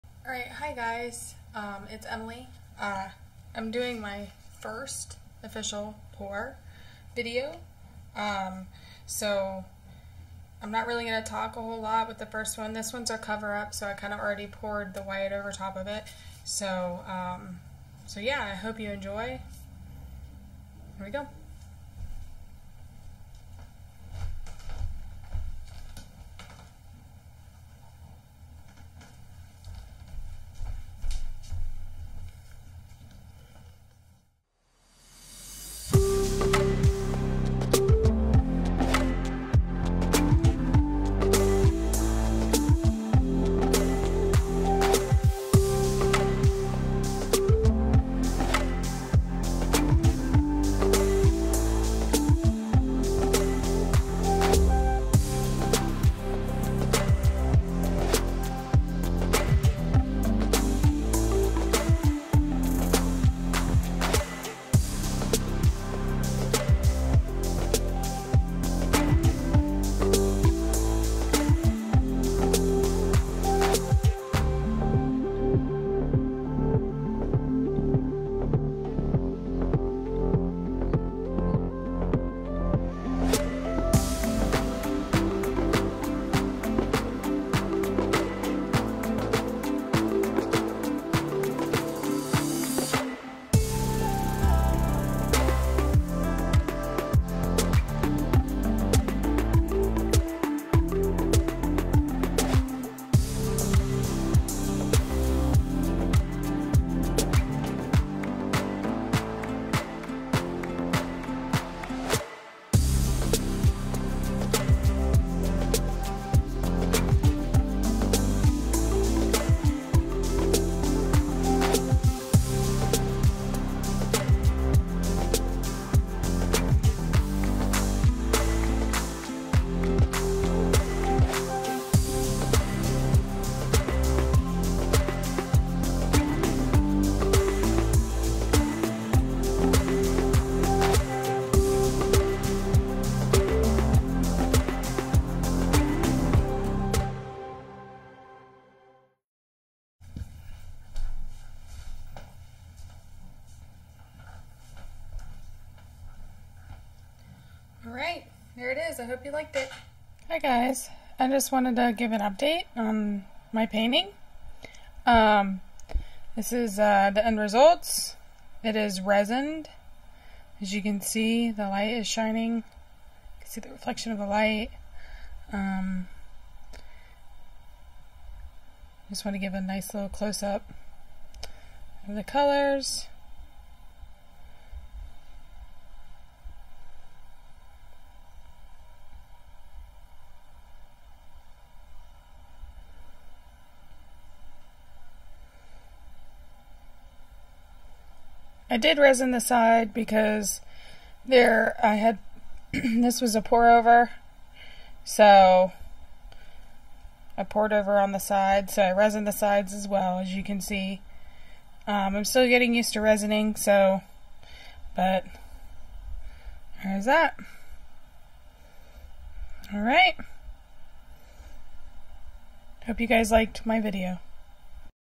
Wells Art and Apparel first video! Emily’s Acrylic Paint Pour Video!
Our first official video! Welcome to Wells Art and Apparel! Come check out how we make our artwork here.  We hope you decide to stay with us!